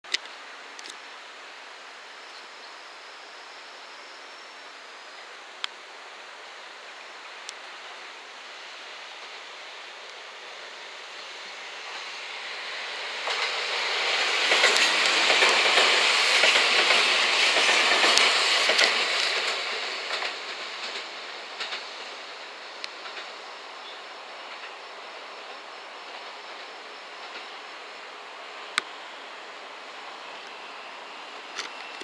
山陽本線 下り 普通列車 227系（2+2両編成） RedWing JR西日本 下関総合車両所 広ヒロ S33+S編成 瀬野付近 000872
山陽本線 下り 普通列車  227系（2+2両編成） RedWing JR西日本 下関総合車両所 広ヒロ  S33+S編成 瀬野付近 00872